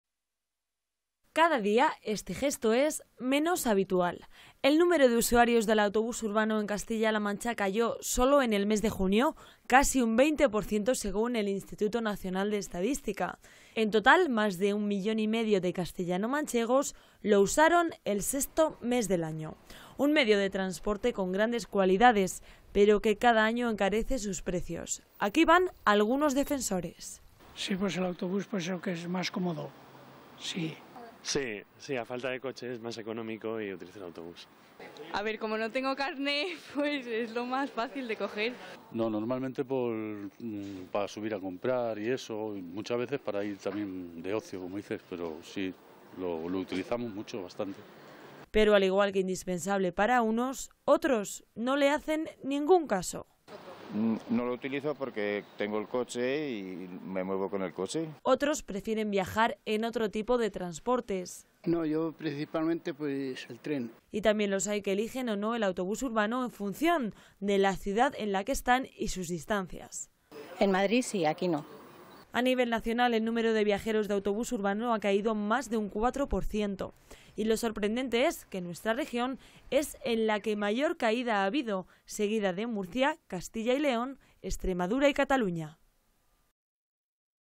Los castellano manchegos han abandonado el hábito de coger el autobús urbano
Un importante número de castellanos manchegos ha abandonado el hábito de coger el autobús urbano. Puede que sea el verano o quizá el aumento de los precios en este tipo de transporte pero lo cierto es que los datos hablan por sí solos.
Cada día este gesto es menos habitual. El número de usuarios del autobús urbano en Catilla La Mancha cayó, sólo en el mes de junio, casi un 20% según el instituto nacional de estadística. En total, más de un millón y medio de castellano manchegos usaron este medio de transporte el sexto mes del año.
Un medio de transporte con grandes cualidades, pero que cada año encarece sus precios. Aquí van algunos defensores.
Pero al igual que indispensable para uno, otros no le hacen ningún caso.
Otros prefieren viajar en otro tipo de transportes, y también los hay que eligen o no el autobús urbano en función de la ciudad en la que están y sus distancias.
A nivel nacional el número de viajeros de autobús urbano ha caído más de un cuatro por ciento, y lo sorprenderte es que nuestra región es en la que mayor caída ha habido, seguida de Murcia, Castilla y León, Extremadura y Cataluña.